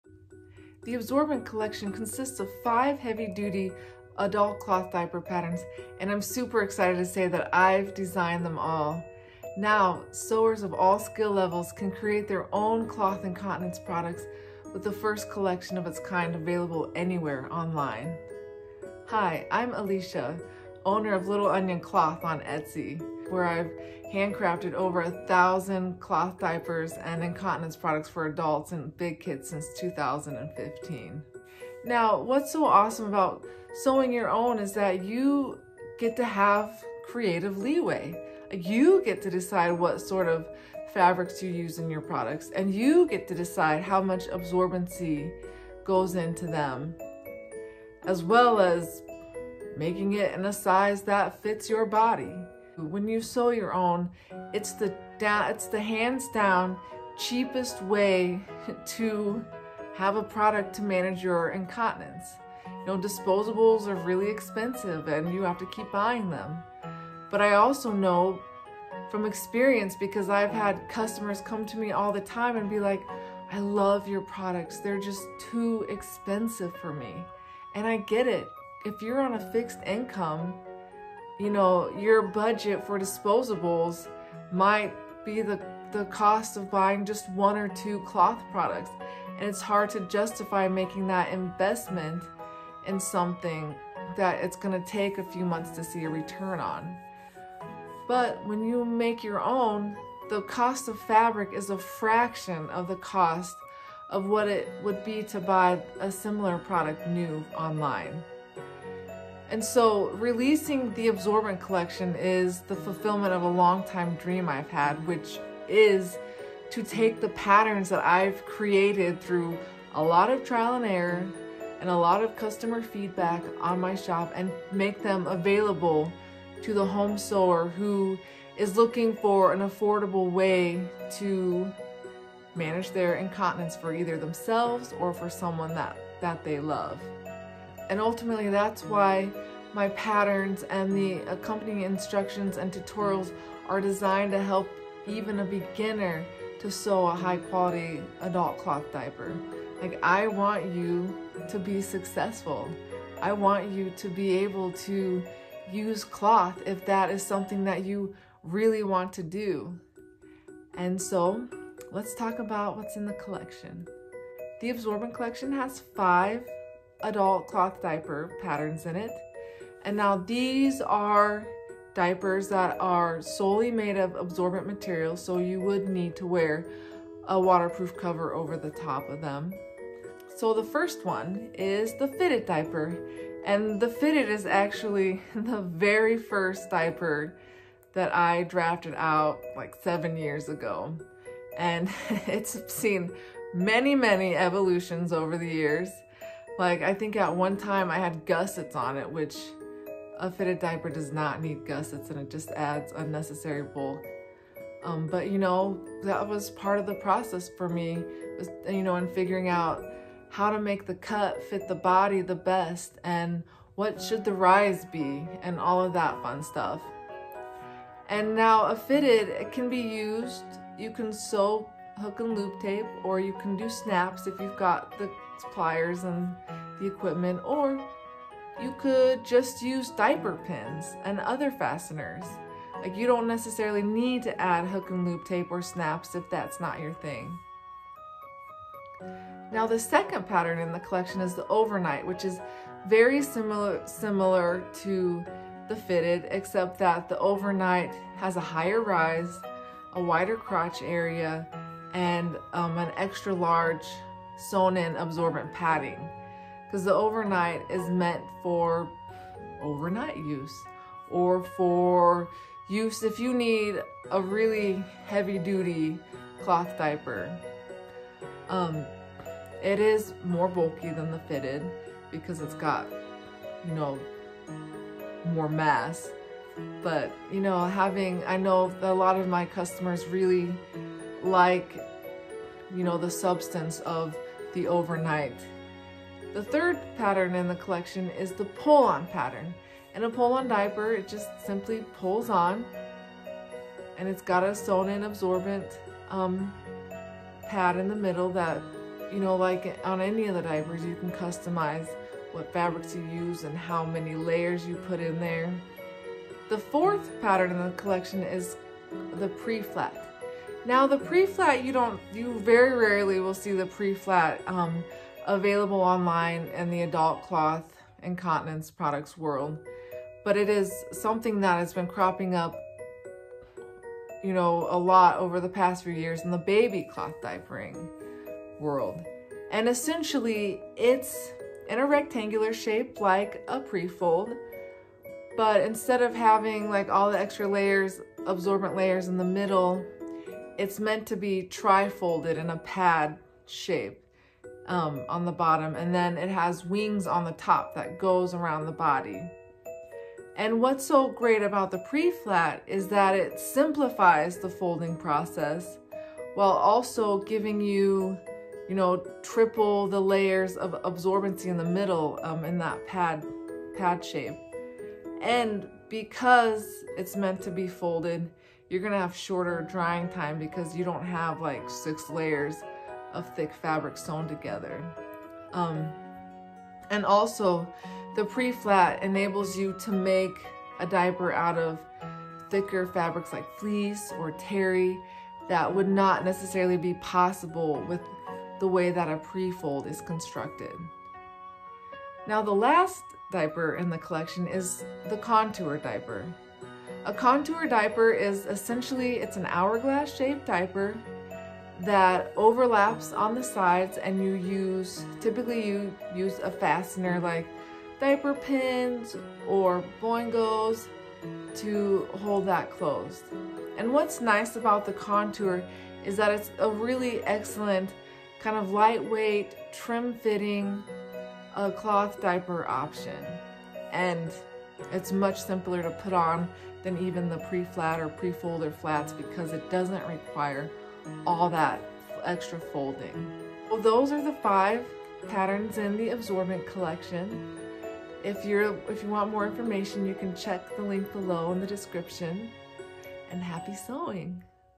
Absorbent Collection of Adult Cloth Diaper Sewing Patterns
Alecia from Little Onion Cloth unveils and discusses a collection of adult cloth diaper sewing patterns that provide extra absorbency protection. Learn about each of the 5 patterns in this collection: Fitted, Overnight, Pull On, Contour, and Preflat.

If cloth diapers are you go-to system for managing your incontinence or that of a loved one, now you can make your own! Sewing your own cloth incontinence products is the cheapest way to manage incontinence - not to mention you get full control over the materials and the fit.

Timestamps:
0:00 Intro
0:31 Creative options
0:52 Save money
1:49 Patterns for home sewer
2:46 Absorbent collection
3:00 Fitted diaper
3:58 Overnight diaper
4:46 Pull On diaper
5:06 Preflat diaper
6:32 Contour diaper

►3 Reasons Why You Need to Switch to Cloth Incontinence Products Now
►How to Add Hook and Loop Tape or Snaps to Adult Cloth Diapers